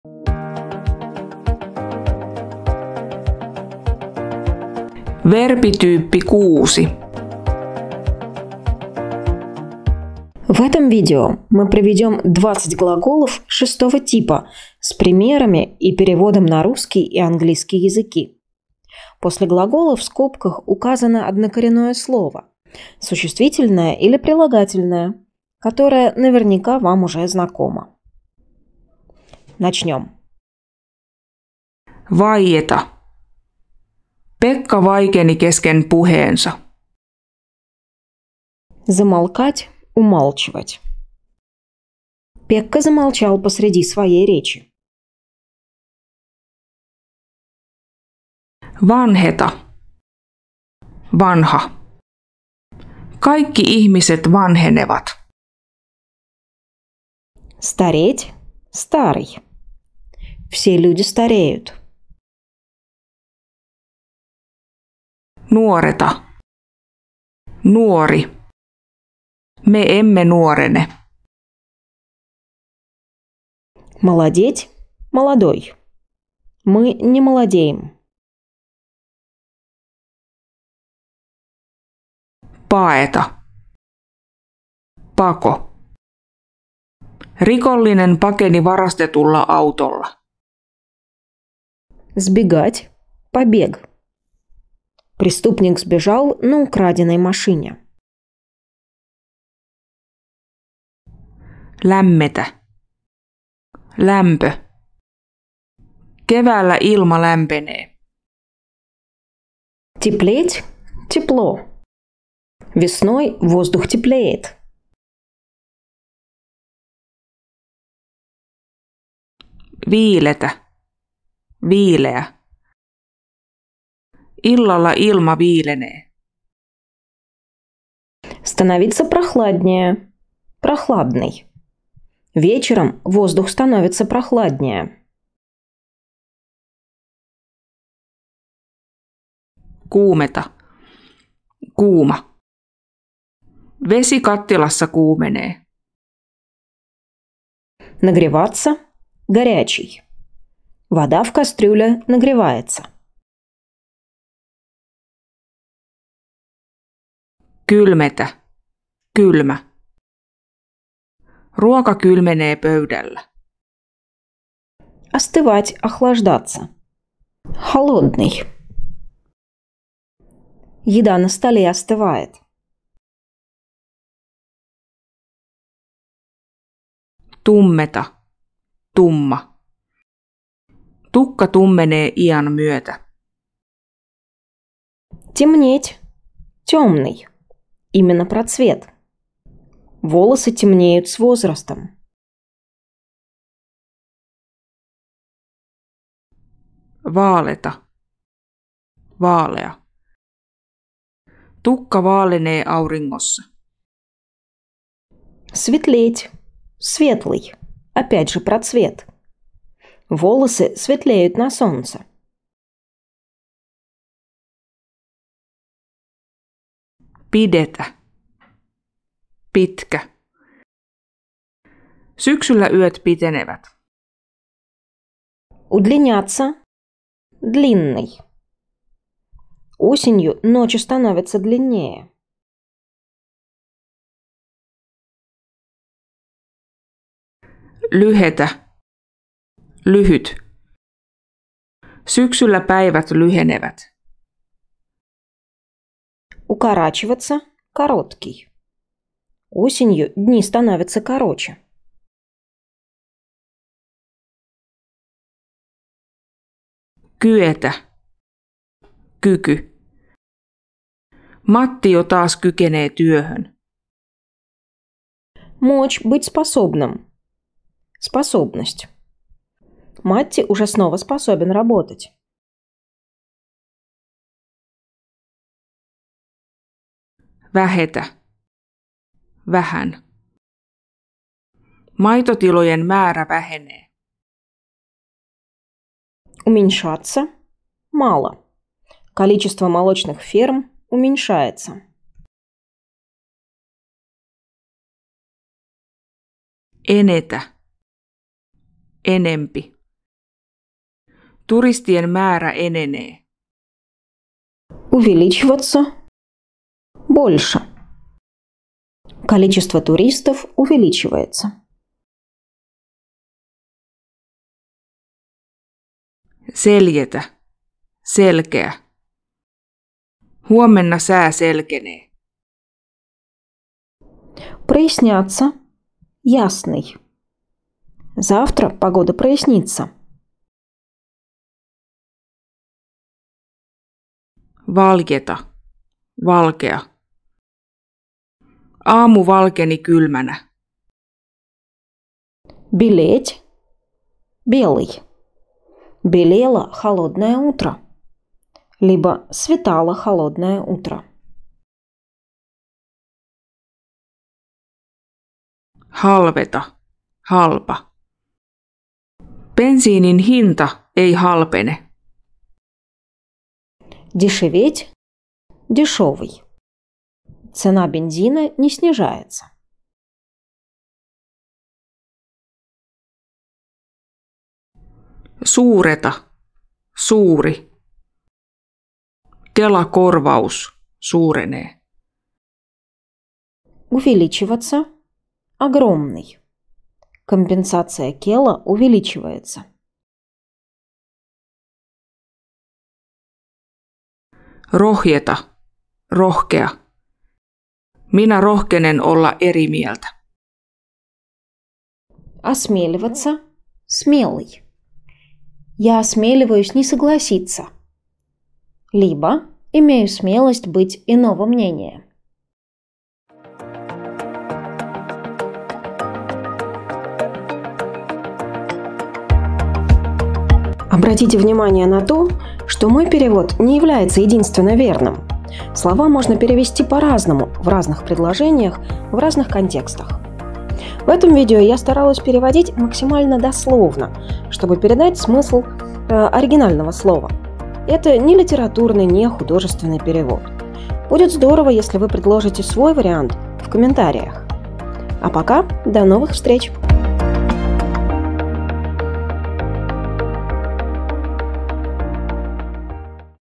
Verbityyppi 6: 20 verbiä + esimerkit
Tässä videossa on 20 verbiä ja esimerkkilauseet niiden kanssa. Verbit ja lauseet ovat käännetty venäjäksi ja englanniksi.